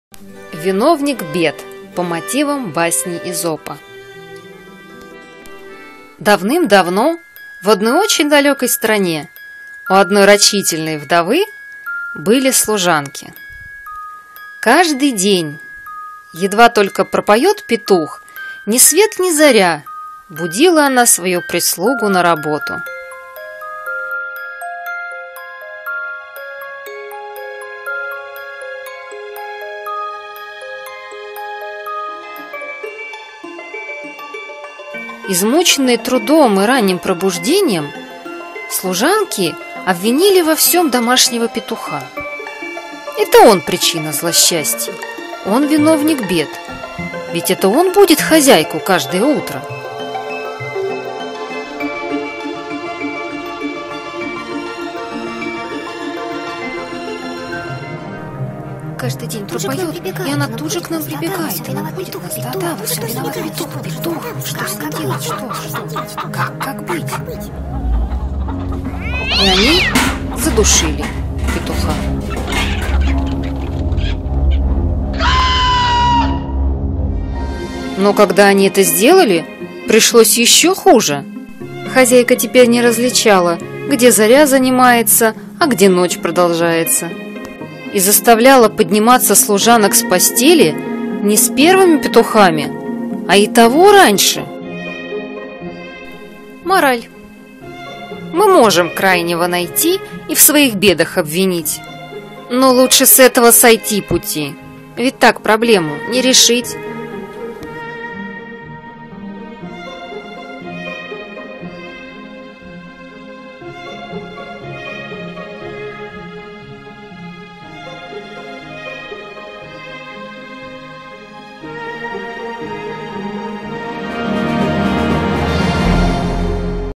"Виновник бед" басня Эзопа ENG SUB Мультфильм со смыслом
По мотивам басни Эзопа "Хозяйка и служанки"
Рекомендовано для семейного просмотра